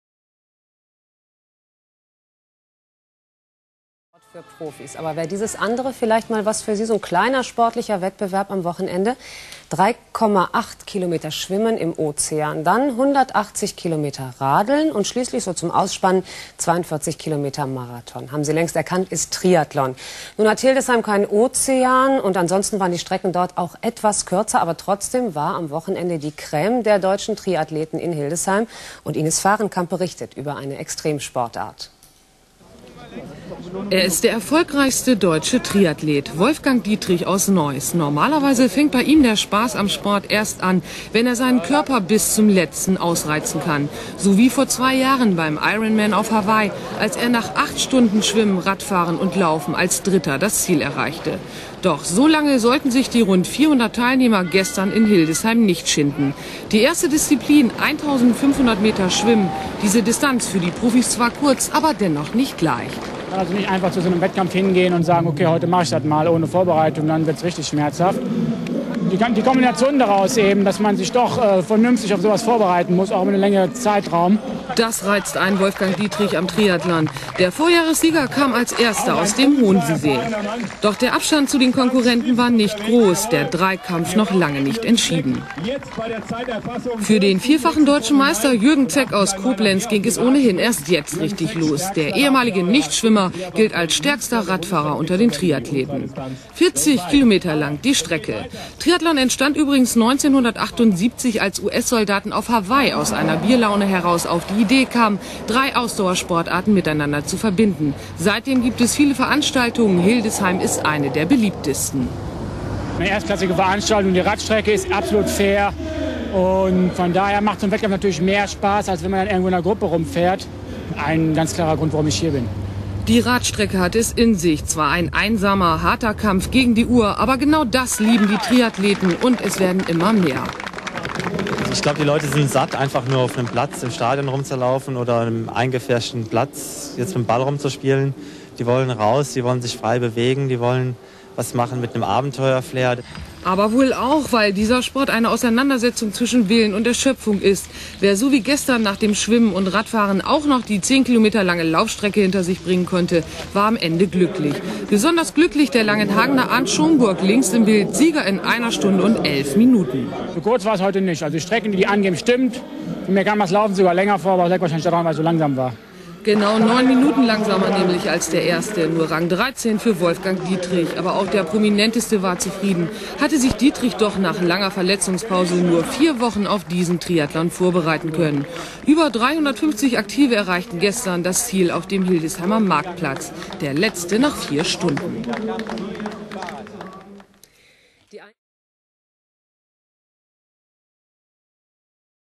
2. Hildesheimer Rose Triathlon '95
2. Bundesliga Nord, Kurz-Triahtlon mit W. Dittrich, J. Zäck, M. Stewart, K. Eckstein, A. Schomburg, I. Estedt, M. Woelk